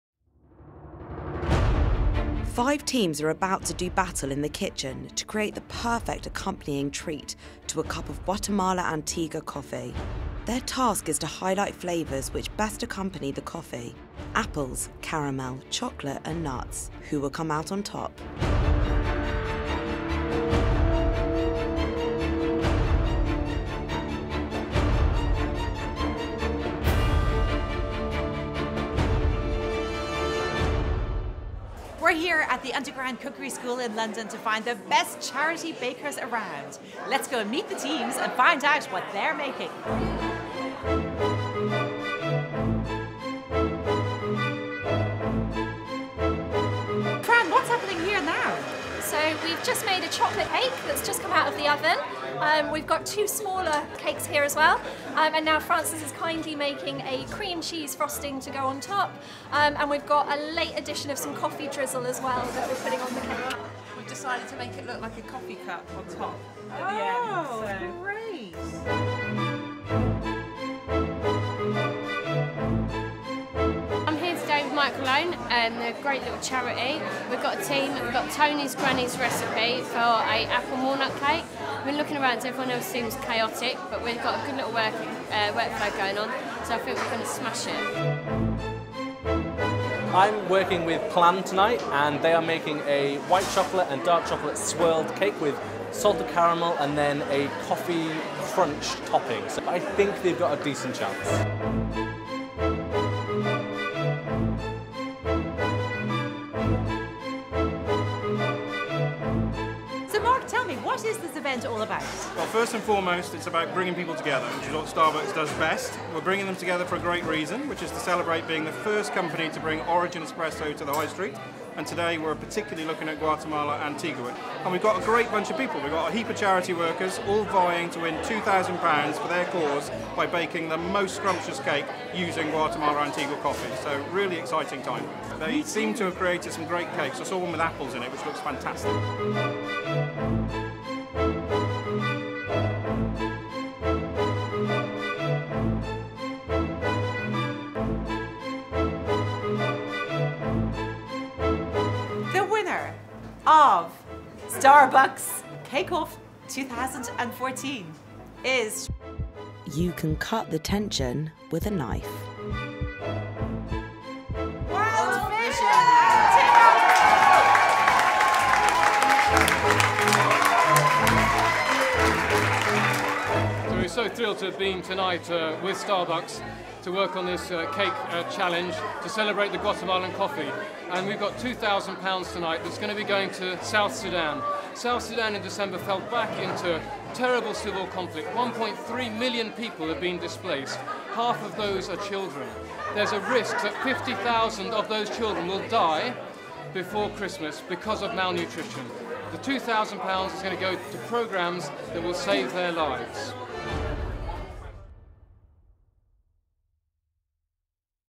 Charity Bake off Final
A Plan UK team went head-to-head with four other international aid charities in a charity Cake Off featuring stars from Masterchef and The Great British Bake Off. The challenge? To impress a panel of celebrity judges with a scrumptious cake in order to bag £2,000 for the charity.

The event was hosted by Plan partner Starbucks UK to celebrate the launch of their single origin Guatemalan Antigua coffee. Guatemala is just one of the 50 countries in which Plan works